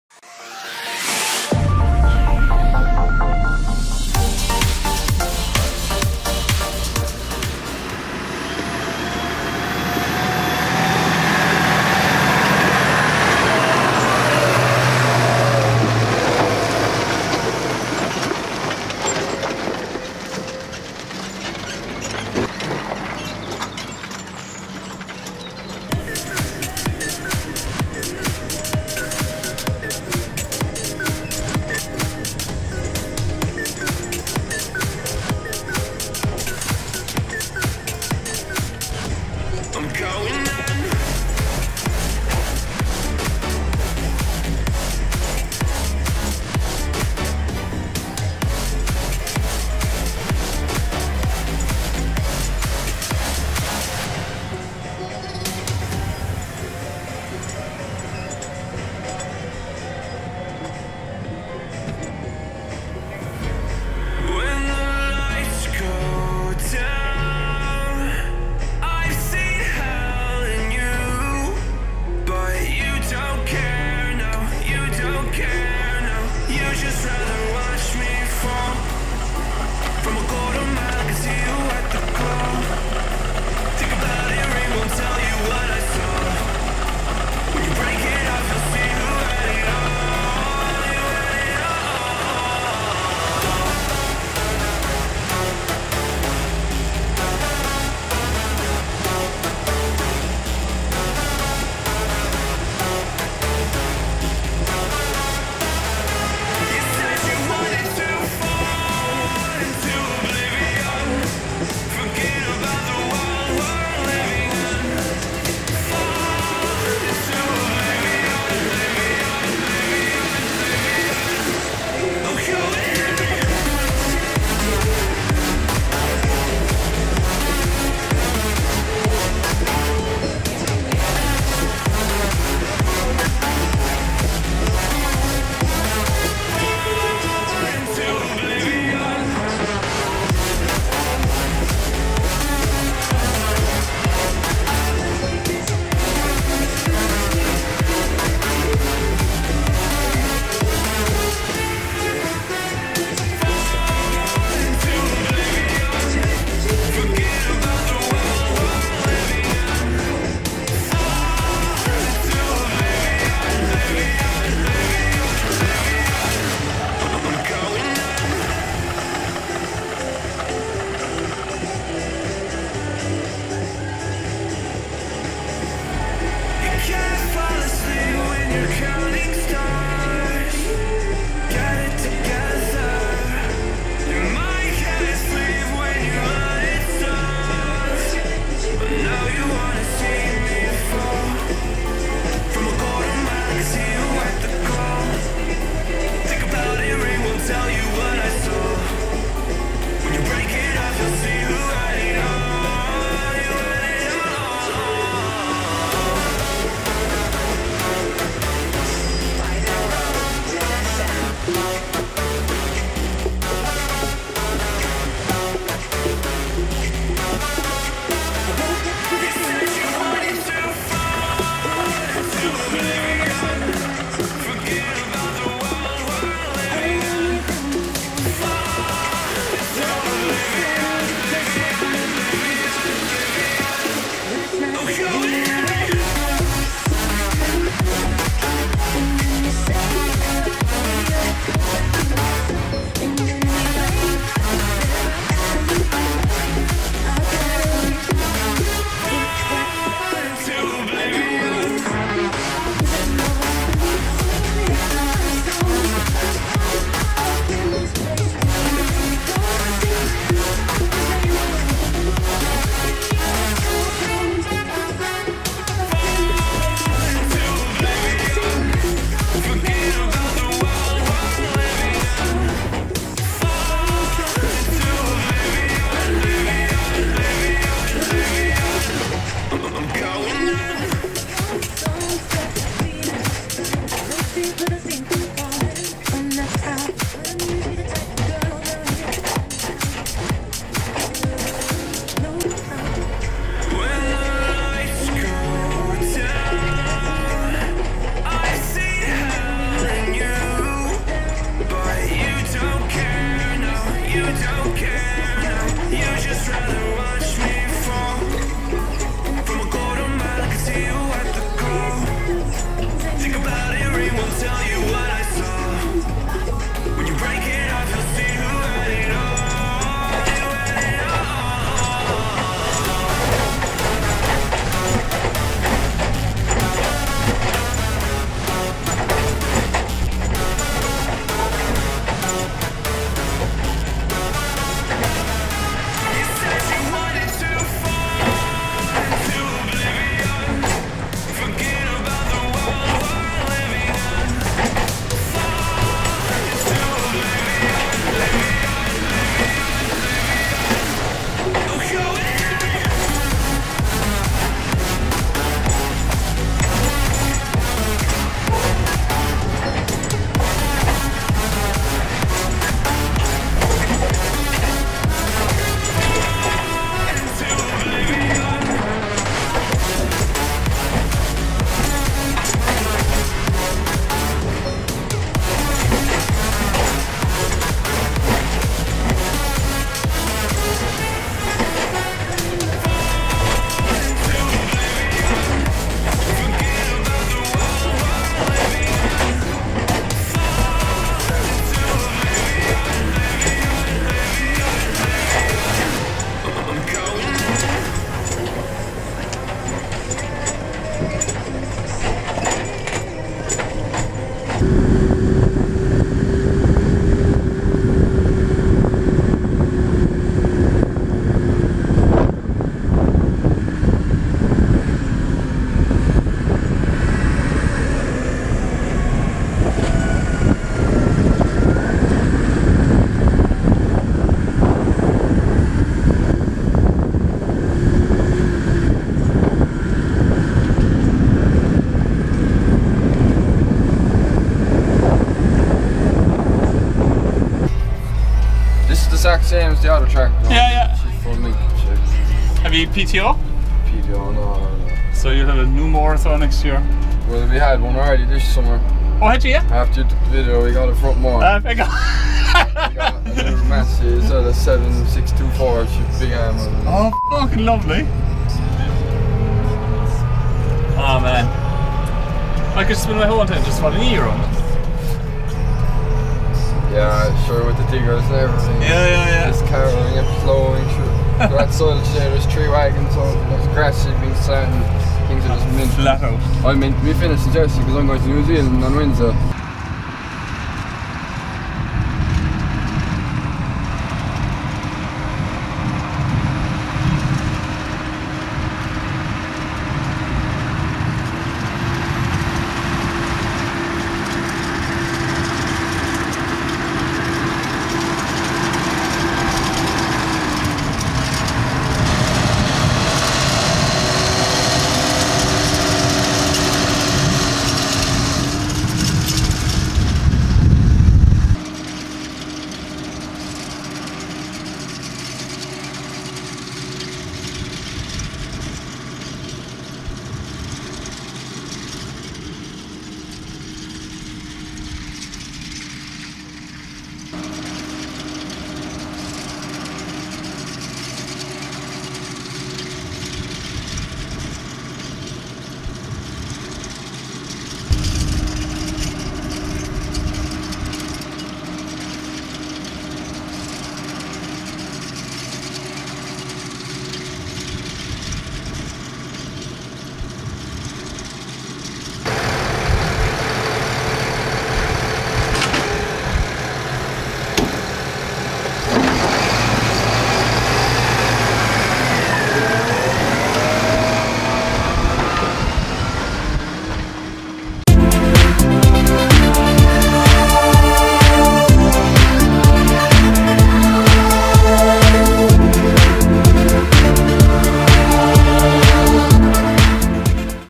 Massey Ferguson 7718 on a Joskin Trailing Shoe Tank HD
Equipment used all linked below.